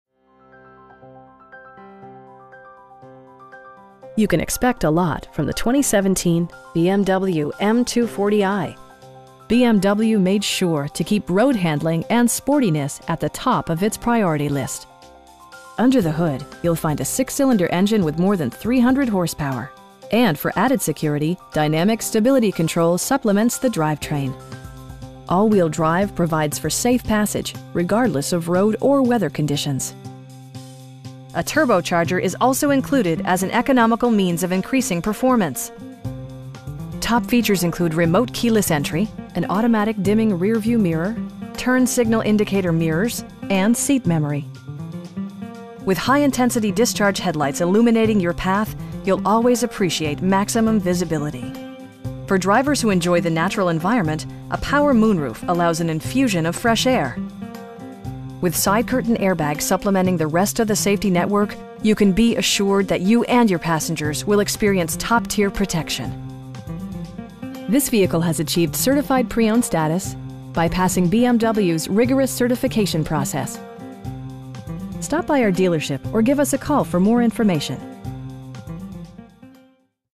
2017 BMW M240i in Fletcher, NC 28732
You can expect a lot from the 2017 BMW M240i This 2 door, 4 passenger coupe still has less than 10,000 miles! BMW made sure to keep road-handling and sportiness at the top of it's priority list. Under the hood you'll find a 6 cylinder engine with more than 300 horsepower, and for added security, dynamic Stability Control supplements the drivetrain. All wheel drive provides for safe passage, regardless of road or weather conditions. A turbocharger is also included as an economical means of increasing performance. Top features include remote keyless entry, a tachometer, variably intermittent wipers, power front seats, an automatic dimming rear-view mirror, turn signal indicator mirrors, power windows, and seat memory. With high intensity discharge headlights illuminating your path, you'll always appreciate maximum visibility. For drivers who enjoy the natural environment, a power moon roof allows an infusion of fresh air. With side curtain airbags supplementing the rest of the safety network, you can be assured that you and your passengers will experience top-tier protection. This vehicle has achieved Certified Pre-Owned status, by passing BMW's rigorous certification process. Our aim is to provide our customers with the best prices and service at all times. Stop by our dealership or give us a call for more information.